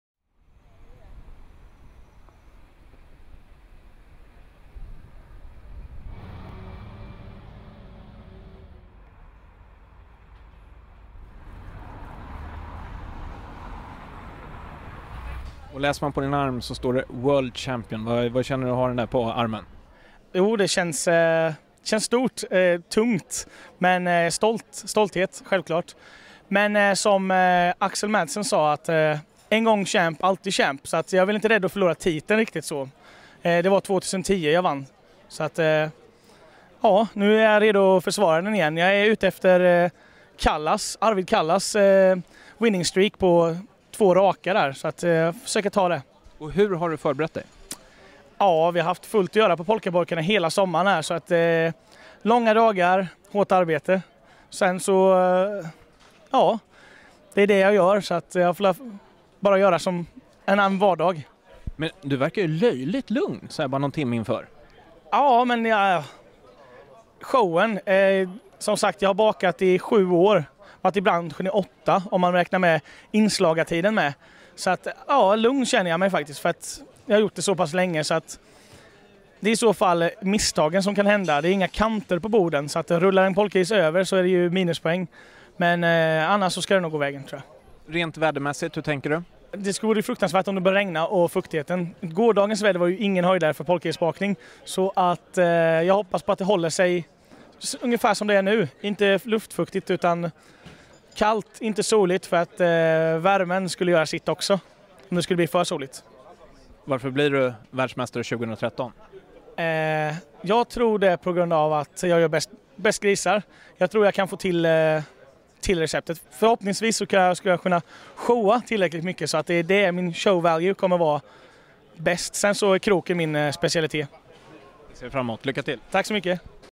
Interview before the Semi-Finals 2013 - Isak Danielsson, Polkapojkarna Gränna
Interview before the Semi-Finals 2013 - Isak Danielsson, Polkapojkarna Gränna - Gränna, Sweden